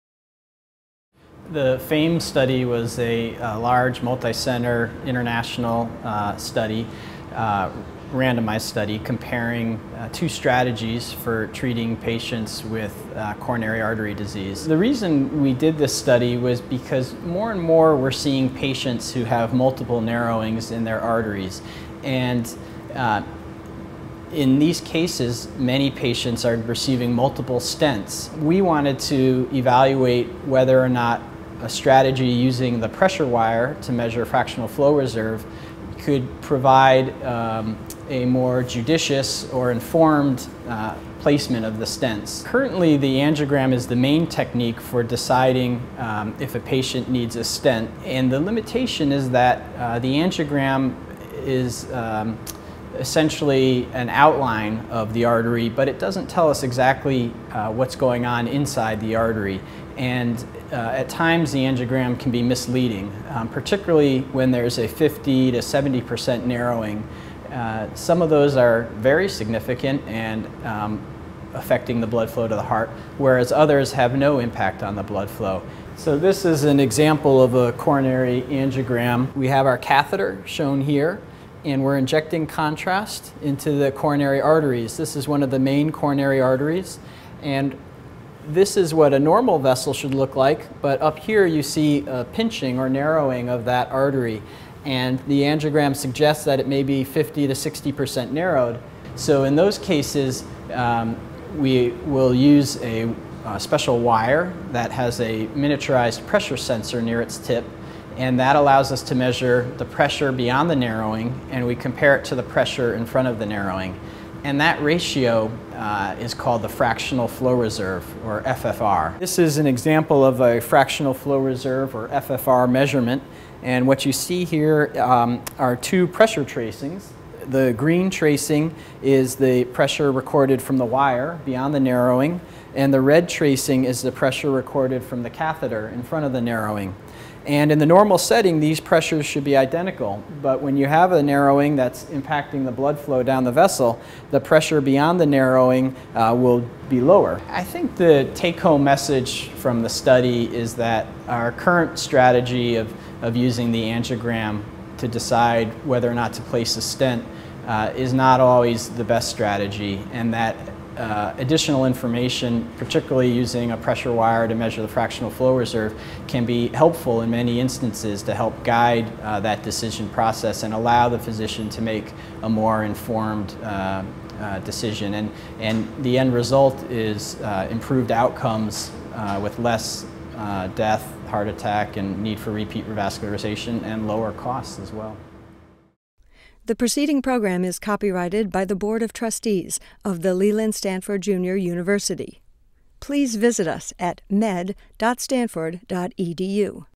New tool could prevent needless stents and save money
Doctors may be implanting too many artery-opening stents and could avoid needless operations if they did more in-depth measurements of blood flow in the vessels to the heart